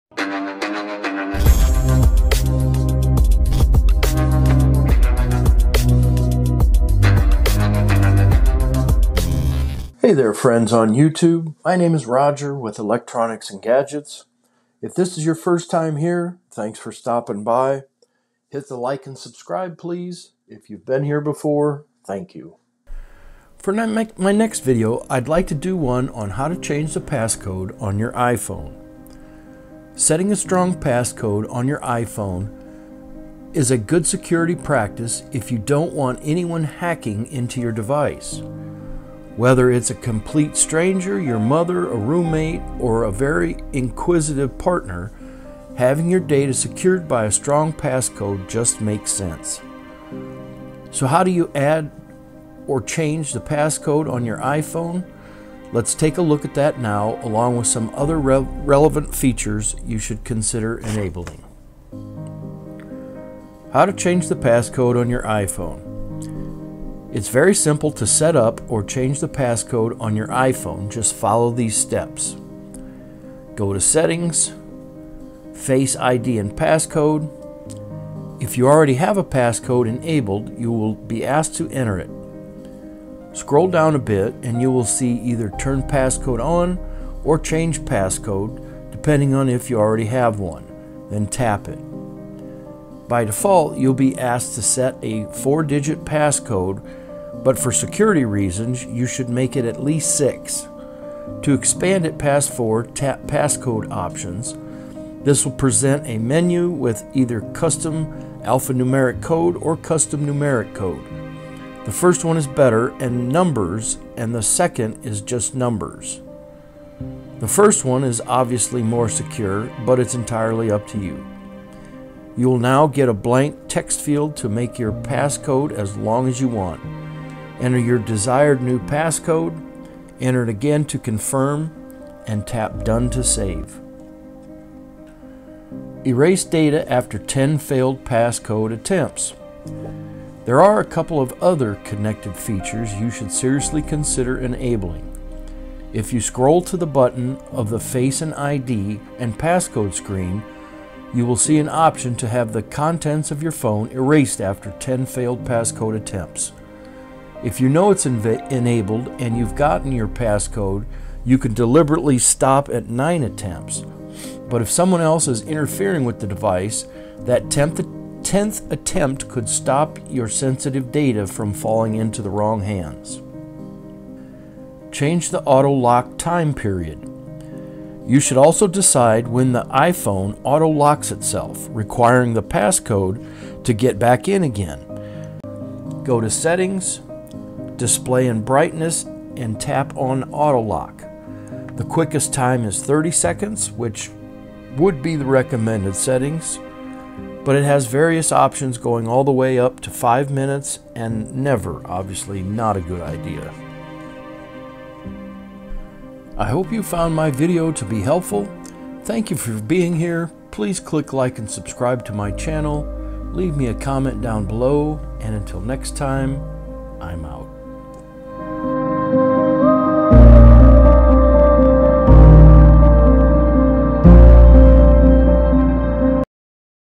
Apple how to change your password electronics and gadgets
In this video, we'll show you how to change your password on your Apple device or laptop. This is a very important step if you want to keep your device or laptop safe, and it's easy to do!

If you've forgotten your password or just need to change it, watch this video and we'll show you how to do it quickly and easily! By following these instructions, you'll be able to keep your device or laptop safe and secure!
Changing your password